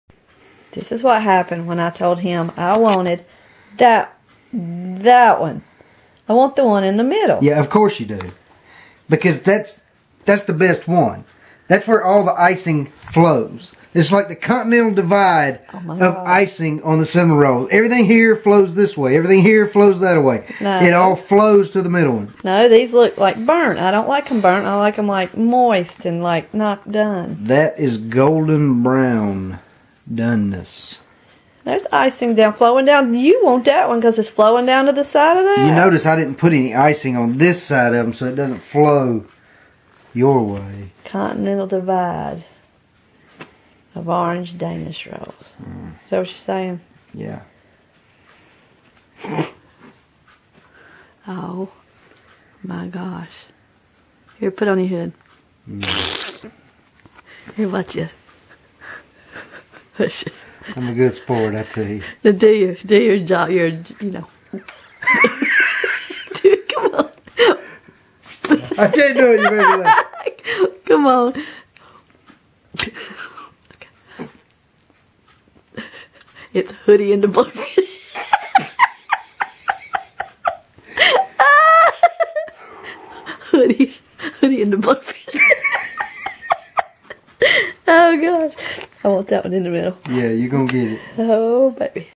Best Brew: "Hoodie and the Blowfish"
When the hubster made us some piping hot Pillsbury Orange Sweet Rolls, he seemed miffed that I wanted the one in the middle where all of the icing flows. But before the brief video ends, we laughingly reconcile when a famous personage shows up! (And yes, we know it's "Hootie and the Blowfish.")